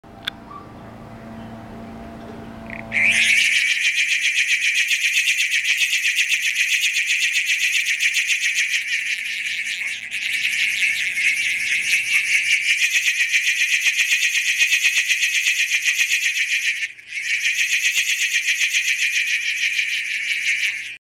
Chinese "Cicada" Toy
Demonstrating a Chinese toy that replicates the sound of an insect.